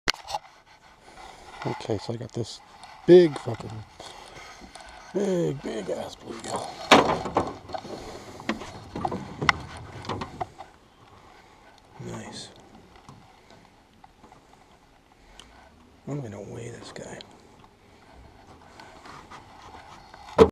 Bluegill caught in the boat. California gold. Country.
BIG ass bluegill. California gold country.